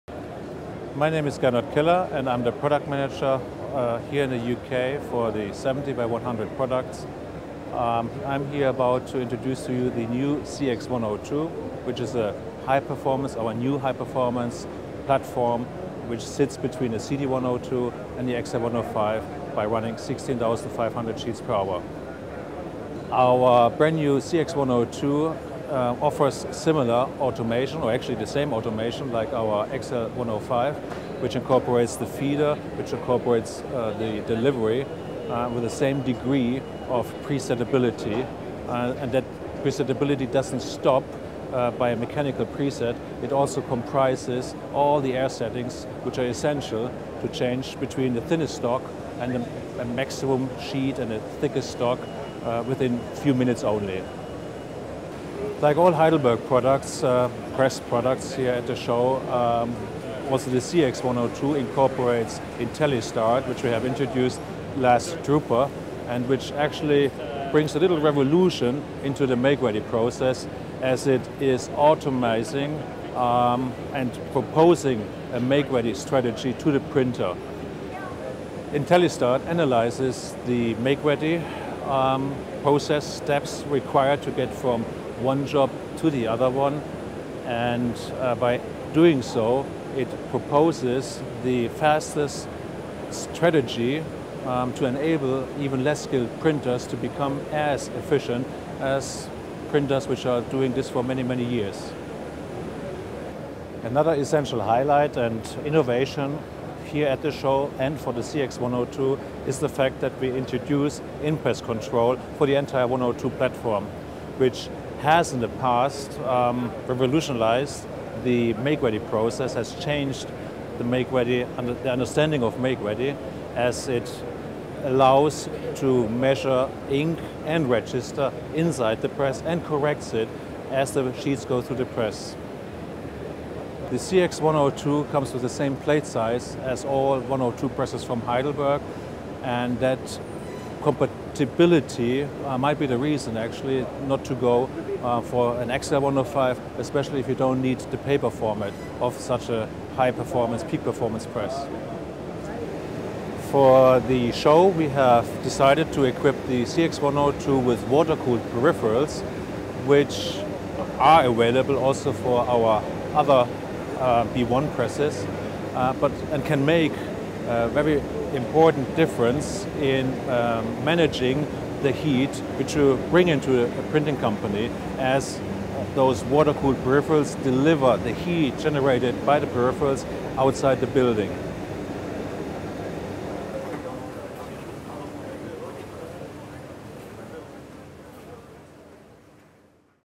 Quick Demo, Heidelberg CX 102 sheetfed offset press
According to Gernot Keller, B1 product manager at Heidelberg UK, the new CX 102 is positioned between the existing CD 102 and XL 105. It is a highly automated 16,500sph machine, configured for fast changeovers beween stocks. Filmed at IPEX 2010